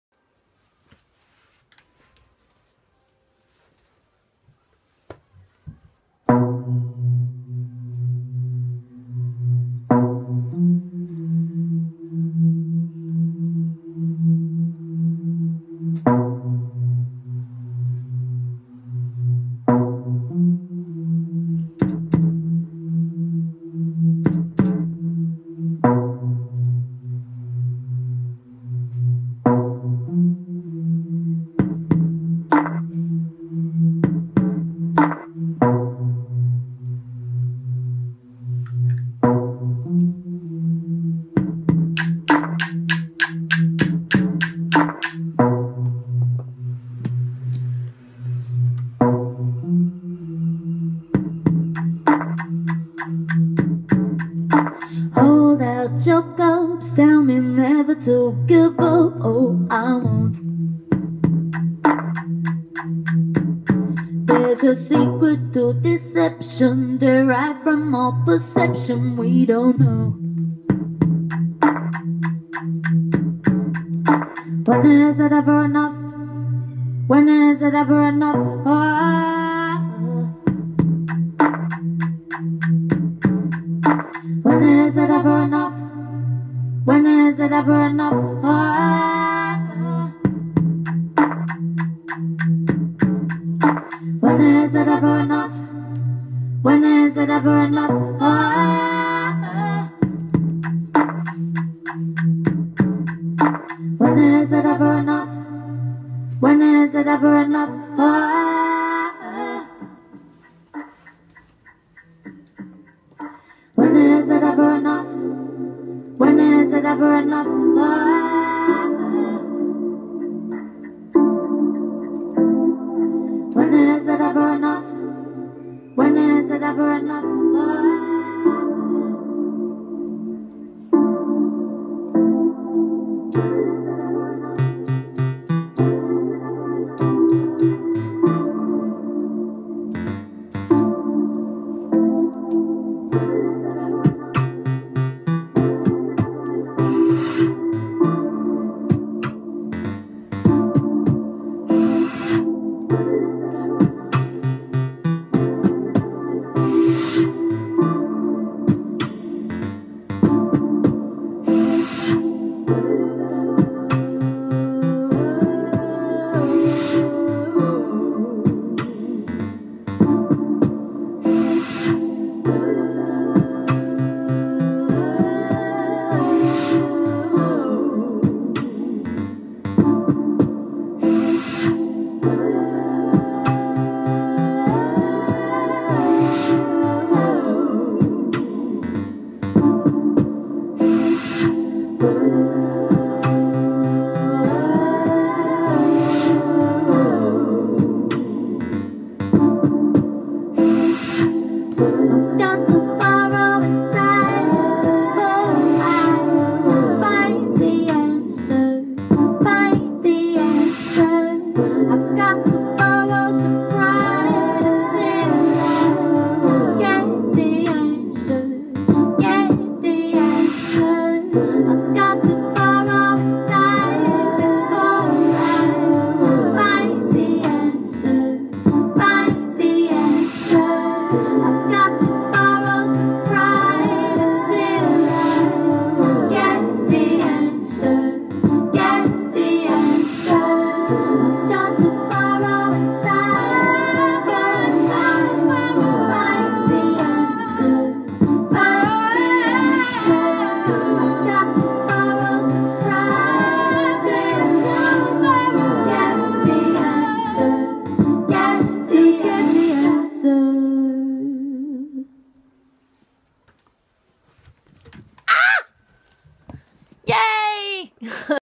Lu'Ami Loop Jam #2 - Blueberry
Every time I practice with my looper I have a lil home jam. I have loads of fun so I thought I would share them with you!